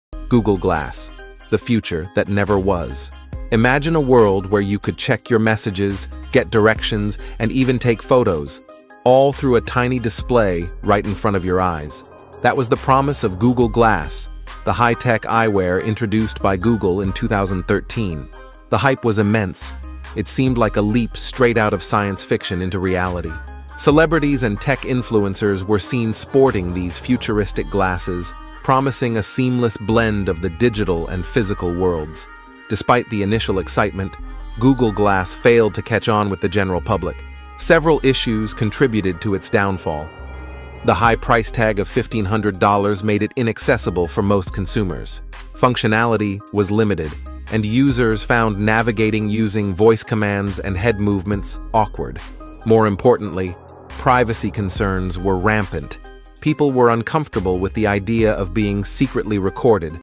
Why Google Glass Failed (And What It Got Right)
Why Google Glass Failed (And What It Got Right) In this video, we journey through the rollercoaster ride of Google Glass, the flashy augmented reality gadget that dazzled the tech scene but ended up face-planting. We dig into the juicy reasons for its flop, including privacy paranoia, social side-eye, and a shortage of uses that didn’t leave consumers scratching their heads. But fear not, it’s not all a tech tragedy! We also shine a spotlight on the snazzy features and tech wizardry that Google Glass brought to the table, setting the stage for the future of wearable wizardry. Discover how its legacy still tickles the evolution of smart glasses and AR today. Join us as we dissect the lessons from Google Glass, what it nailed, and how these gems can shape the next wave of wearable wonders.